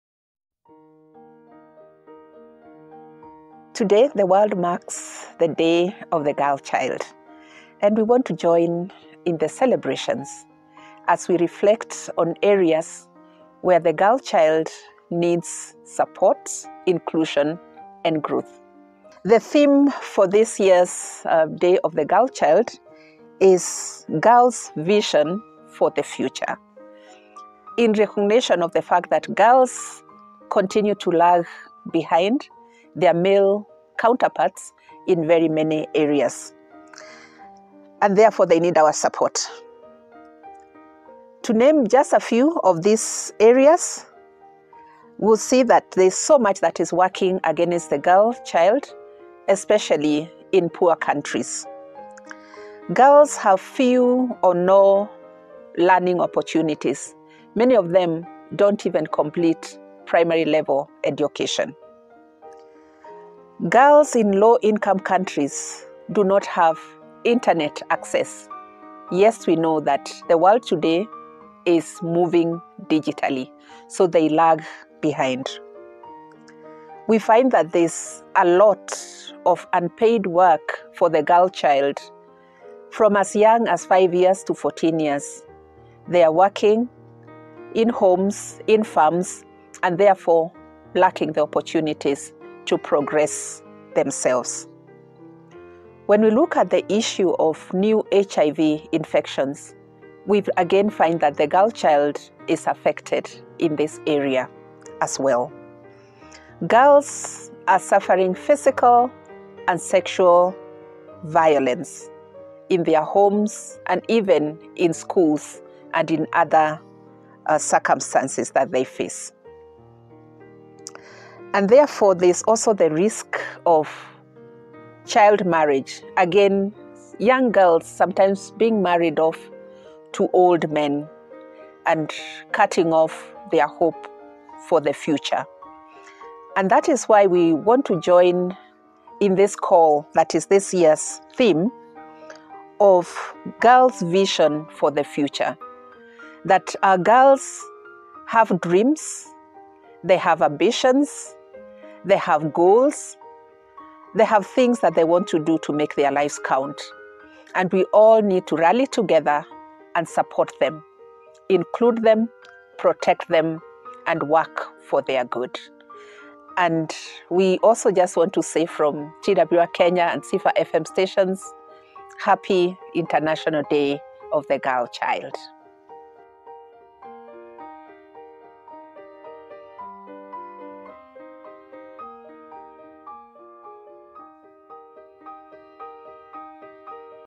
International Day of the Girl Child
Today, we celebrate the strength, resilience, and limitless potential of every girl.
As we mark International Girl Child Day, let's continue to champion equal opportunities for girls around the world, ensuring they have access to education, healthcare, and the freedom to dream.

Every girl deserves a future where her voice is heard, her rights are respected, and her aspirations are within reach. Together, we can build a world where every girl child thrives and shines. Happy International Girl Child Day!